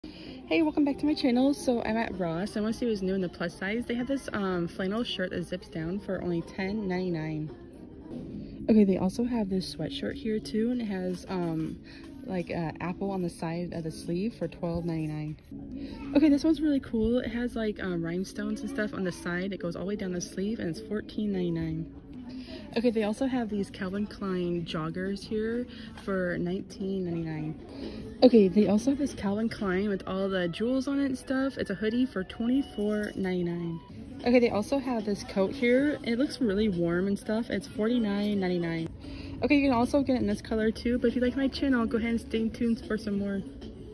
💙 PLUS SIZE CLOTHING AT ROSS DRESS FOR LESS‼️
Come with me to Ross Dress For Less as I found so many cute styles and new finds! So many great options including shirts, tank tops, Tommy Hilfiger shirts, Calvin Klein shirts, Bebe shirts and so much more!